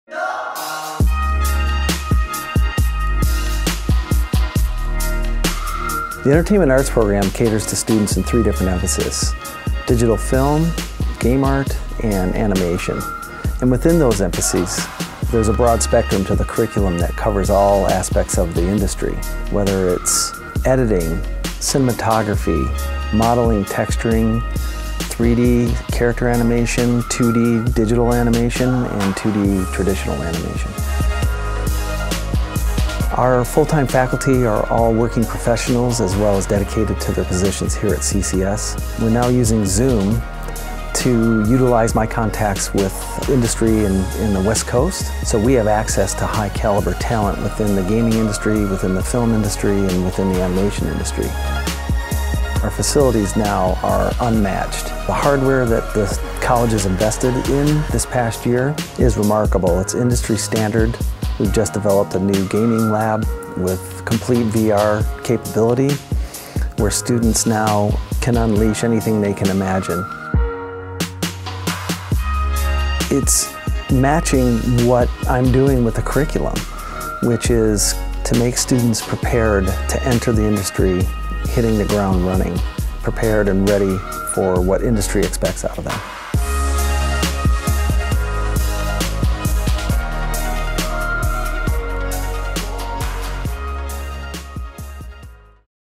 Entertainment Arts | CCS
Storytelling is at the heart of what artists do. The mediums may differ, but whether you’re venturing into animation, video games or digital film, it takes the best to help you become the best.

Entertainment Arts at CCS prepares you to join the next generation of storytellers with Hollywood insiders who show you how to master the latest software and equipment used in the entertainment industry.